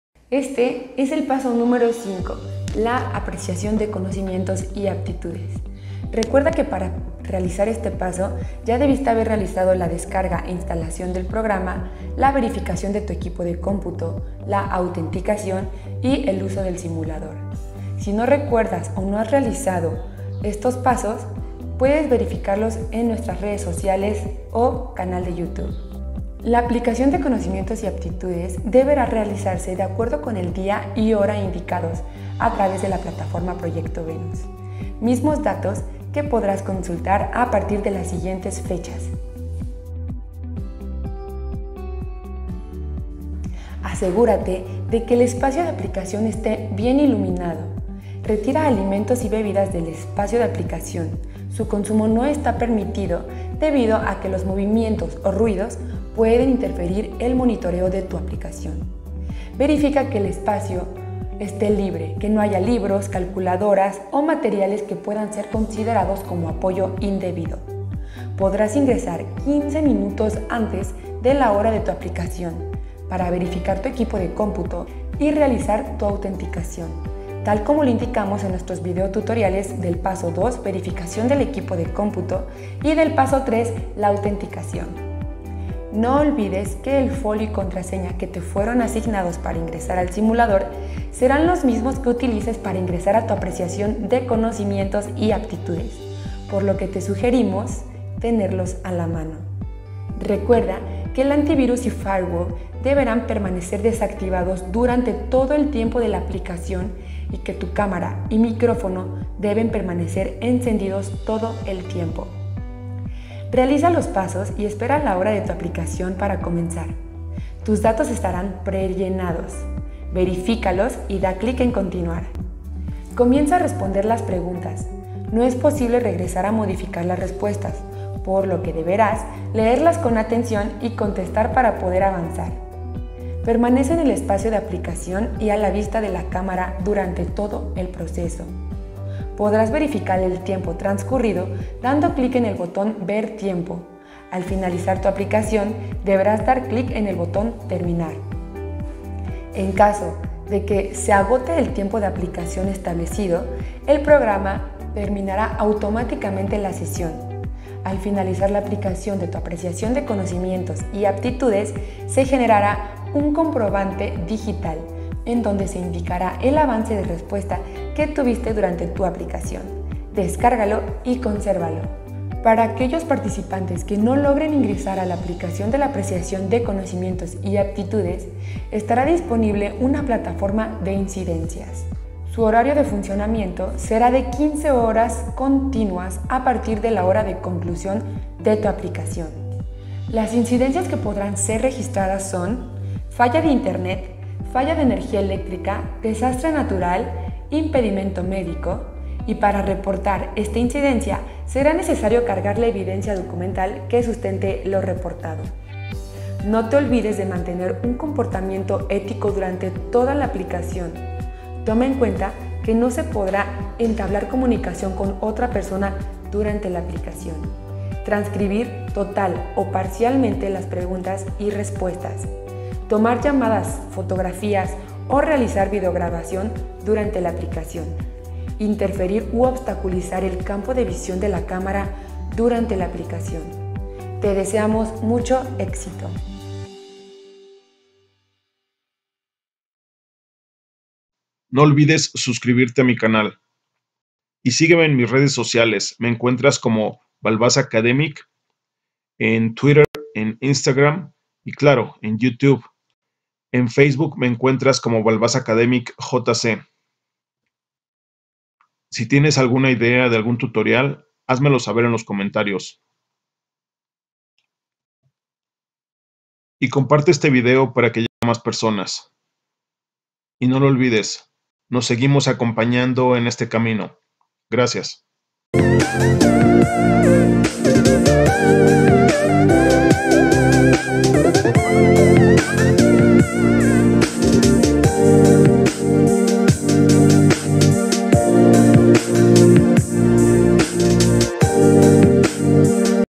PASO 5 APRECIACIÓN DE CONOCIMIENTOS Y APTITUDES (USICAMM).
Balvas Academic: EL DÍA DE HOY EN ESTE VÍDEO TE COMPARTO EL PASO 5 APRECIACIÓN DE CONOCIMIENTOS Y APTITUDES (USICAMM).